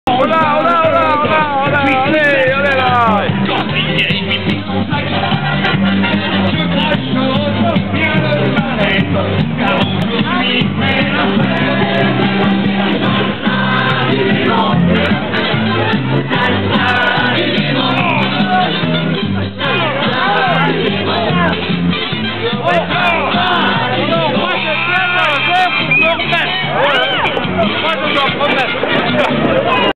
Jalhay Carnaval 26 février 2012 Groupe du Baron Anges et Démons
Un magnifique cortège sous le brouillard +5° mais de l'ambiance à revendre.